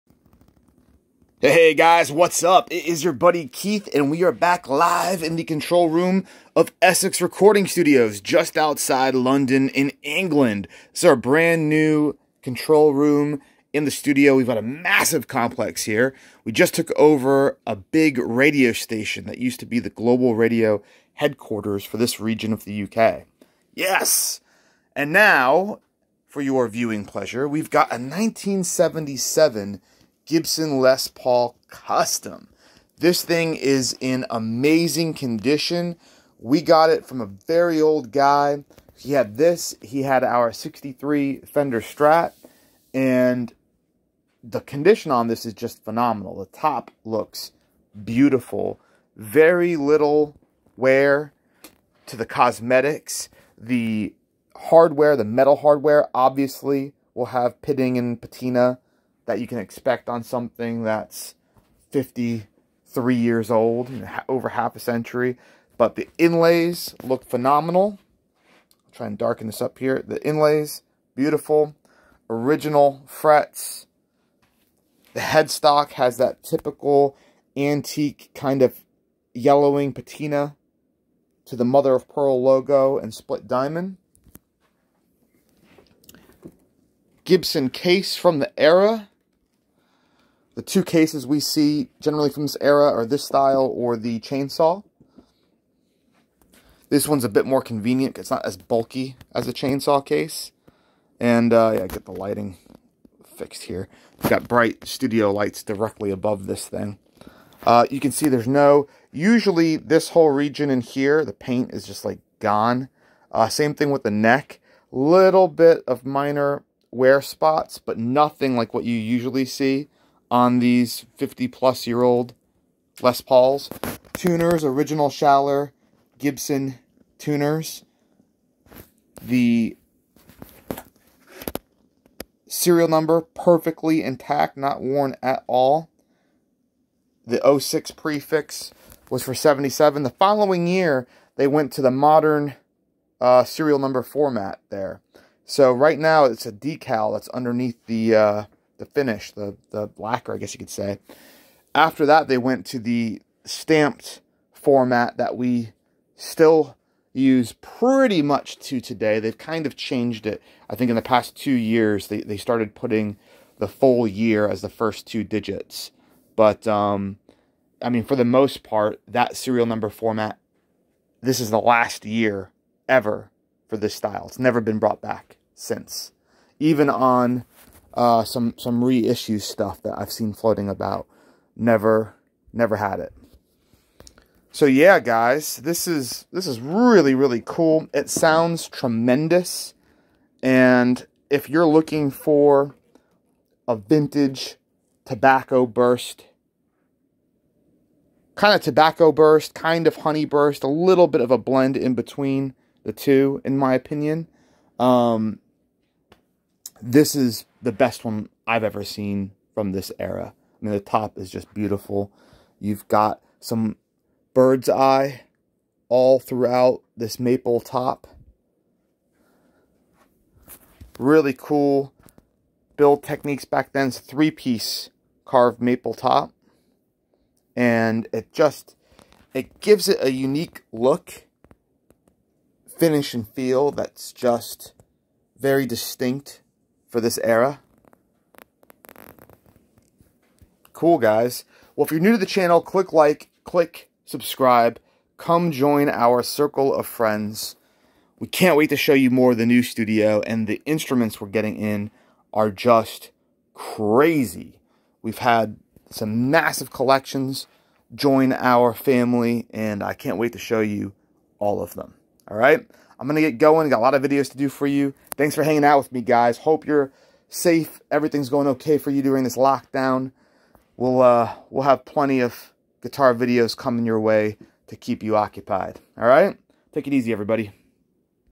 1977 Gibson Les Paul Custom Tobacco Burst at Essex Recording Studios - Up Close Guitar Video Review
1977 Gibson Les Paul Custom Tobacco Burst at Essex Recording Studios - Up Close Guitar Video Review